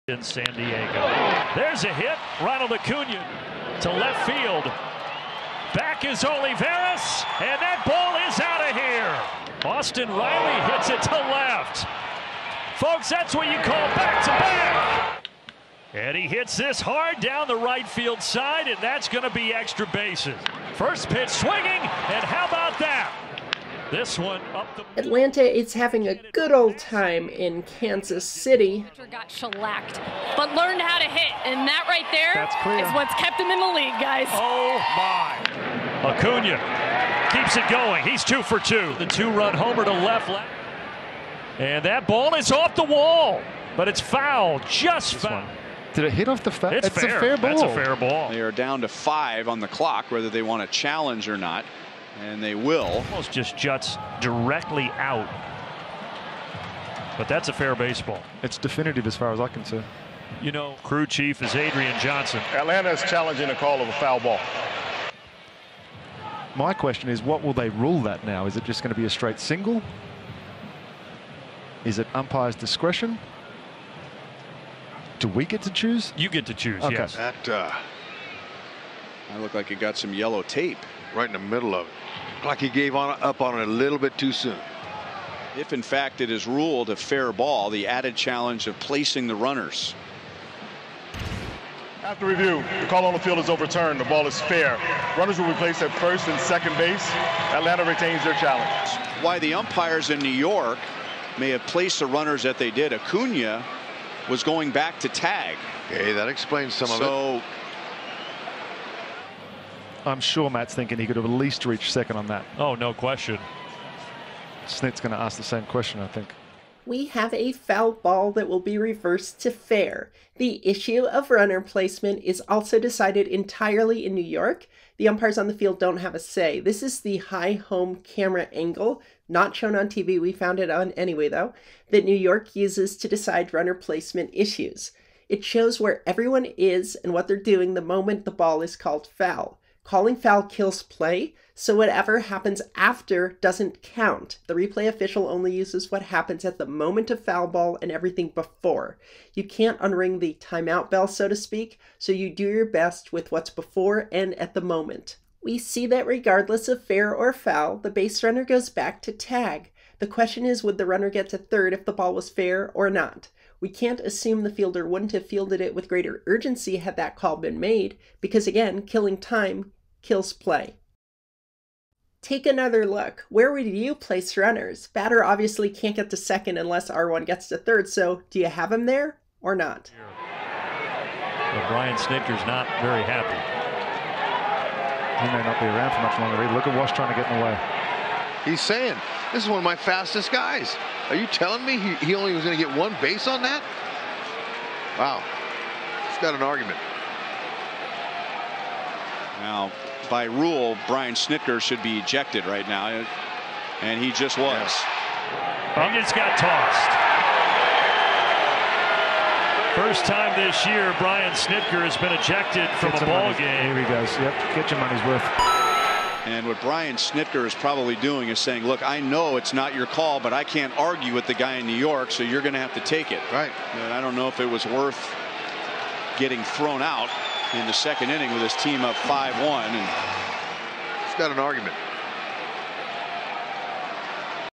E12 - Brian Snitker Ejected After Replay Review Runner Placement Call Overturns Umpire's Foul Ball
3B Umpire Manny Gonzalez ejected Braves manager Brian Snitker for arguing a Replay Official's runner placement call in Kansas City after overturning Gonzalez's foul ball in left field to fair. Report

Atlanta batter Matt Olson hit a fly ball to the left field corner where 3B Umpire Gonzalez ruled it a foul ball. Upon challenge by Snitker, Replay Review overturned the call to fair ball and placed baserunner R1 Ronald Acuna at second base and batter-runner Olson at first base.

Replays via the high home camera angle show where each player was when the ball was called foul by Gonzalez—which creates a dead ball and kills play. The replays indicate that baserunner Acuna went back to first base to tag, since the fly ball could theoretically have been caught by Royals left fielder Edward Olivares. It wasn't, and Acuna began running toward second base as Gonzalez called the foul ball.

The Replay Official deemed that had Gonzalez called fair from the get-go, that Acuna would have only made it to second base due to his strategy of going back to tag up when the ball fell to the ground, and that batter-runner Olson's advancement was limited by Acuna being placed at second base.